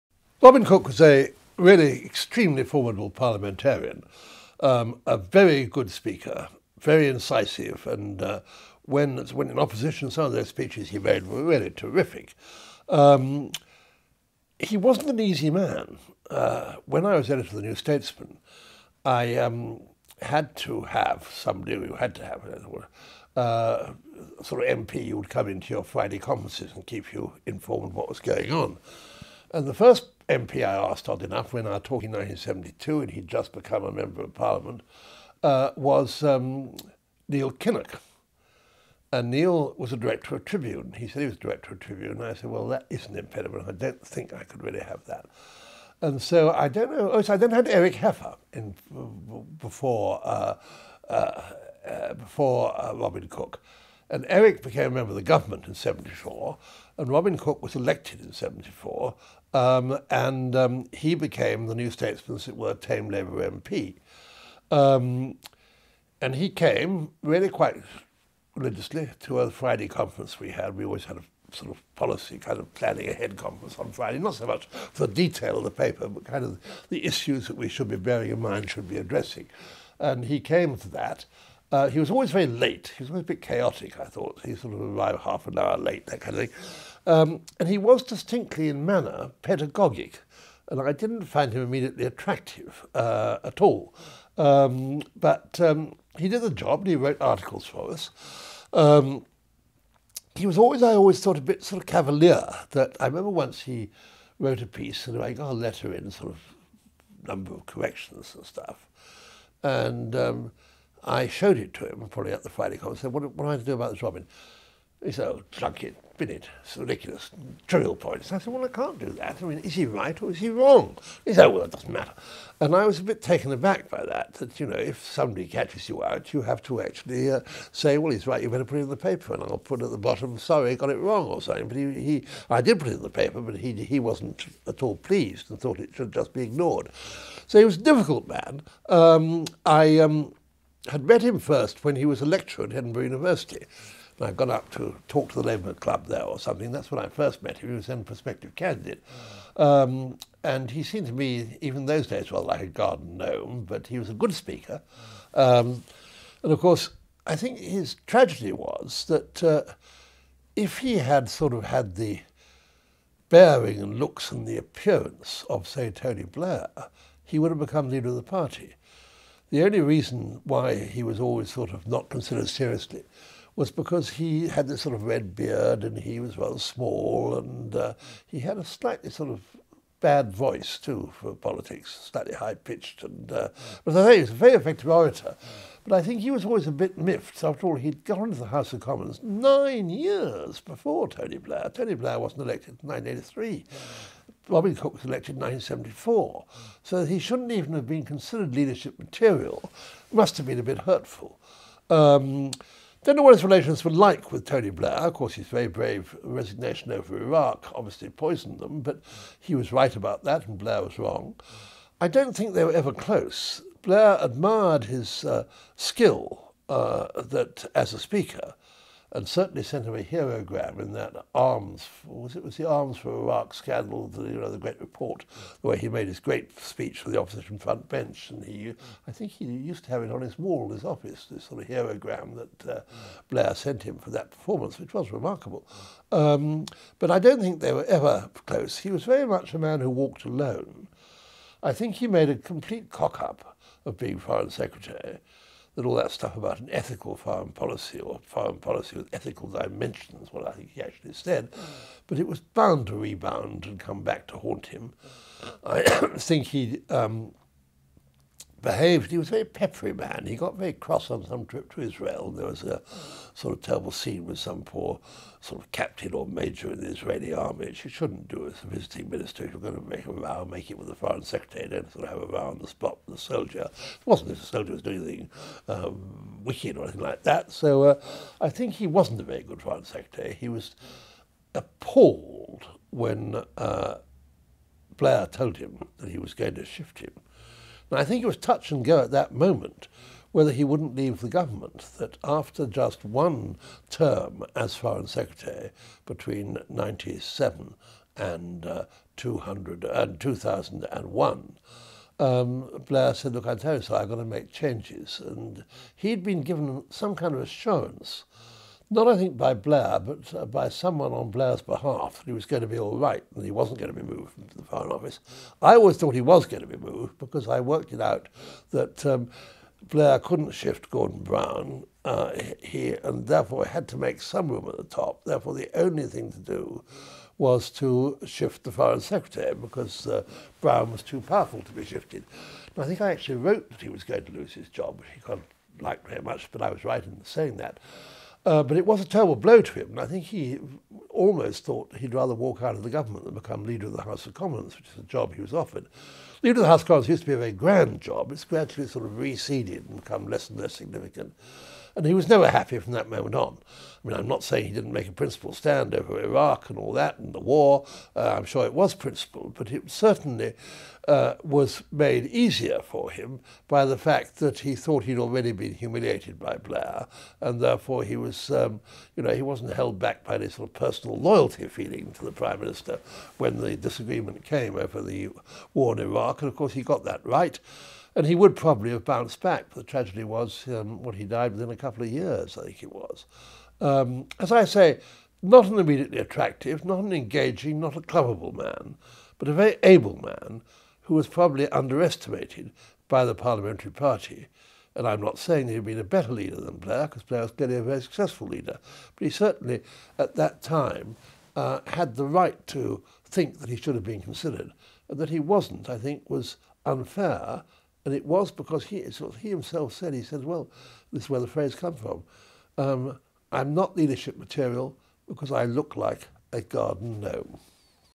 Anthony Howard - Robin Cook was not leadership material (29/41)
The prominent British political observer, Anthony Howard (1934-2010) reported on global political issues for over 40 years for 'The Guardian', 'The Sunday Times' and 'The Observer', and was editor of the 'New Statesman' and 'The Listener'. He received a CBE in 1997. [Listener: Christopher Sykes; date recorded: 2008]

TRANSCRIPT: Robin Cook was a really extremely formidable parliamentarian, a very good speaker. Very incisive and, when in opposition, some of those speeches he made were really terrific. He wasn’t an easy man. When I was editor of the 'New Statesman', I had to have somebody… or you had to have... a sort of MP who’d come into your Friday conferences and keep you informed on what was going on. And the first MP I asked, oddly enough, we’re now talking 1972, and he’d just become a member of parliament, was Neil Kinnock. And Neil was a director of 'Tribune'. He said he was a director of 'Tribune', and I said, 'Well, that is an impediment and I don’t think I could really have that'. And so I don’t know… so I then had Eric Heffer in, before Robin Cook. And Eric became a member of the government in '74, and Robin Cook was elected in '74, and he became the 'New Statesman', as it were, tame labour MP. And he came, really quite religiously, to every Friday conference we had. We always had a, sort of, policy, kind of planning-ahead conference on Friday, not so much for detail in the paper, but kind of the issues that we should be bearing in mind, should be addressing.

And he came to that. He was always very late. He was always a bit chaotic, I thought. He’d sort of arrive half an hour late, that kind of thing. And he was distinctly, in manner, pedagogic, and I didn’t find him immediately attractive at all. But he did the job and he wrote articles for us. He was always, I always thought, a bit sort of cavalier that… I remember once, he wrote a piece and I got a letter in, sort of, number of corrections and stuff. And I showed it to him, probably at the Friday conference, and said, 'What am I to do about this, Robin?' And he said, 'Oh, junk it... bin it. It’s ridiculous. Trivial point'. So I said, 'Well, I can’t do that. I mean, is he right or is he wrong?' He said, 'Oh, well, it doesn’t matter'. And I was a bit taken aback by that, that, you know, if somebody catches you out, you have to actually say, well, he’s right, you'd better put it in the paper and I’ll put at the bottom, sorry, got it wrong, or something. But he… I did put it in the paper, but he wasn’t at all pleased, and thought it should just be ignored. So he was a difficult man.

I had met him first when he was a lecturer at Edinburgh University, and I’d gone up to talk to the Labour Club there or something. That’s when I first met him. He was then a prospective candidate. And he seemed to me, even in those days, rather like a garden gnome, but he was a good speaker. And of course, I think his tragedy was that if he had, sort of, had the bearing and looks and the appearance of, say, Tony Blair, he would have become leader of the party. The only reason why he was always, sort of, not considered seriously, was because he had this sort of red beard and he was rather small and he had a slightly, sort of, bad voice, too, for politics. Slightly high-pitched and… but as I say, he was a very effective orator. But I think he was always a bit miffed, because after all, he’d gone to the House of Commons nine years before Tony Blair. Tony Blair wasn’t elected until 1983. Robin Cook was elected 1974. So he shouldn’t even have been considered leadership material. It must have been a bit hurtful.

I don’t know what his relations were like with Tony Blair. Of course, his very brave resignation over Iraq obviously poisoned them, but he was right about that, and Blair was wrong. I don’t think they were ever close. Blair admired his skill as a speaker, and certainly sent him a hero-gram in that arms… was it? was the Arms for Iraq scandal, you know, the great report where he made his great speech for the opposition frontbench. And he used… I think he used to have it on his wall in his office, this, sort of hero-gram that Blair sent him for that performance, which was remarkable. But I don’t think they were ever close. [...]

to read the remaining part of the transcript.